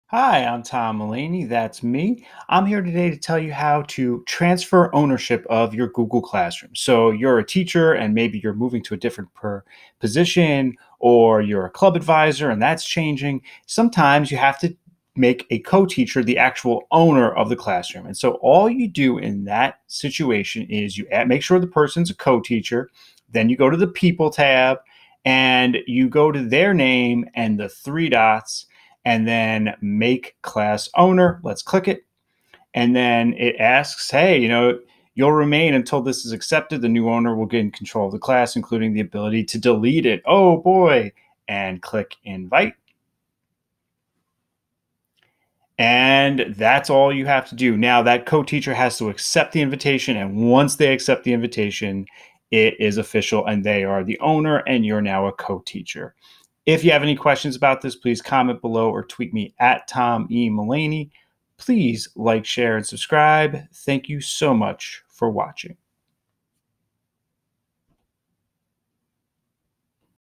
Google Classroom - Transfer Class Ownership
Do you need to make another teacher the owner of your Google Classroom? It's so easy!